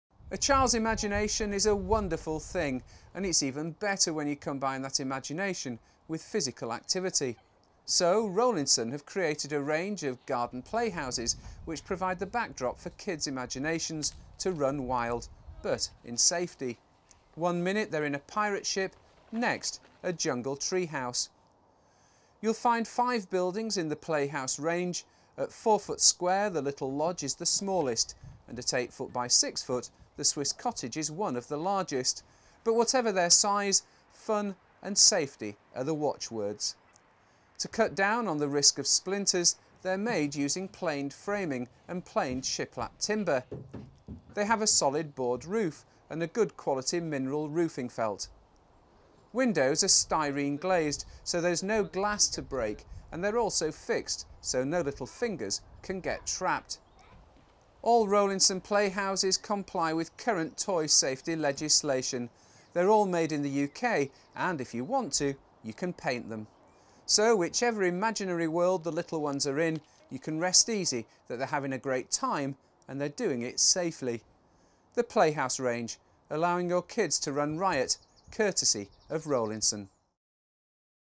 Features of the Rowlinson Playhouse Range
With a broad selection of playhouses available, the Rowlinson range of small lodges are perfect for endless hours of child's play and a great way to keep your children entertained within the garden.